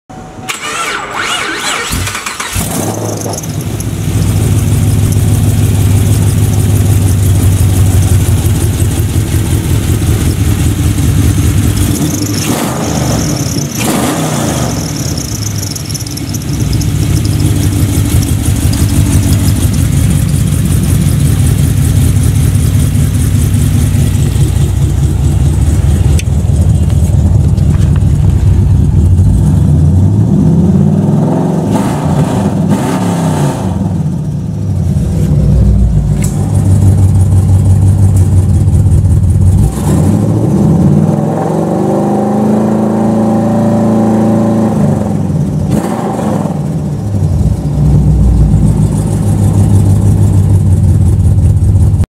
2010 Camaro SS L99 Automatic 102K Miles
What's Included:
        Complete L99 Engine w/ 100K Miles (Intake to Oil Pan)
        6-Speed 6L80 Automatic Transmission w/ Crossmember
        Complete Engine Wiring Harness w/ ECM Computer